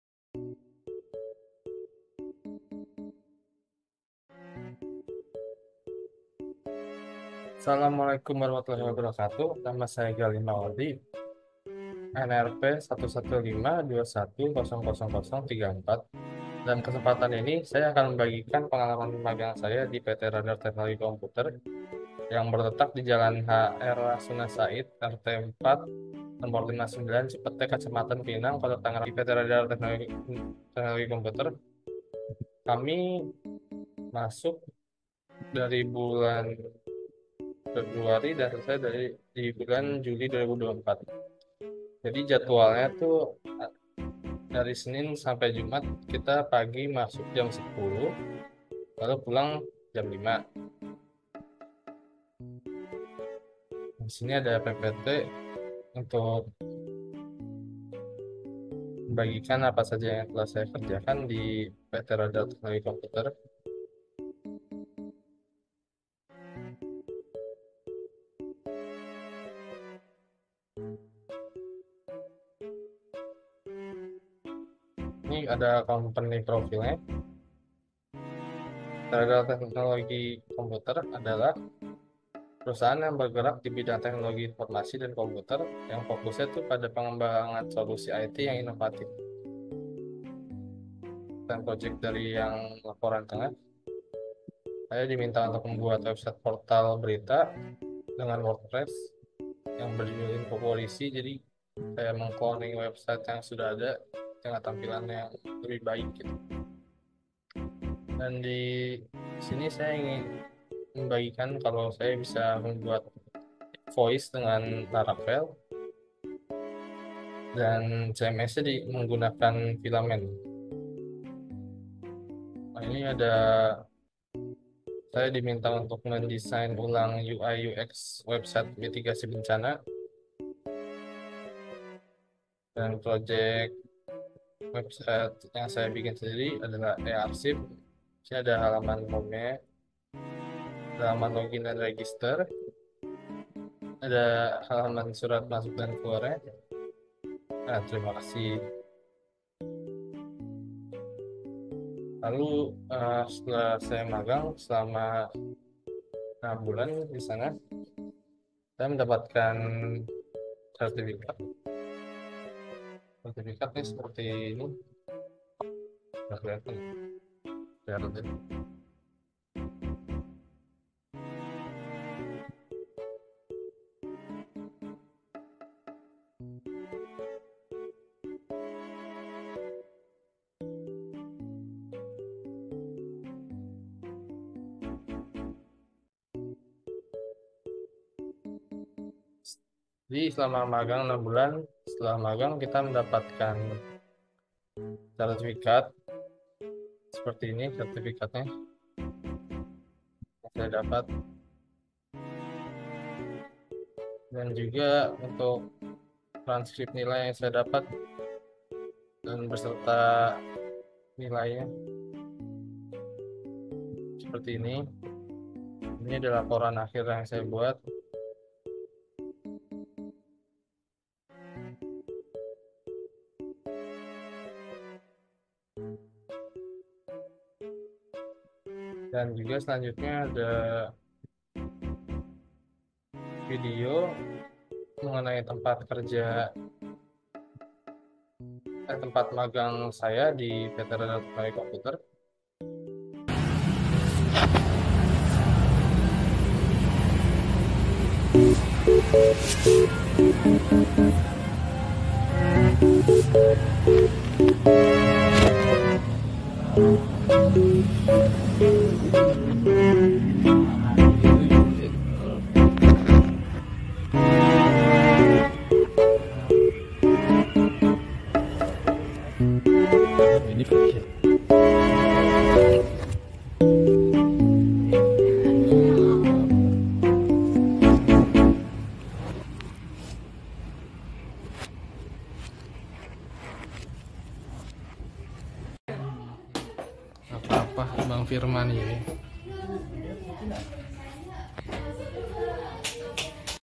Magang Bersertifikat di PT. Radar Teknologi Komputer
BENTUK KEGIATAN PEMBELAJARAN

Magang Bersertifikat

Semester 6 / Tahun Akademik 2023/2024

PESERTA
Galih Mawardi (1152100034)

NAMA MITRA
PT. Radar Teknologi Komputer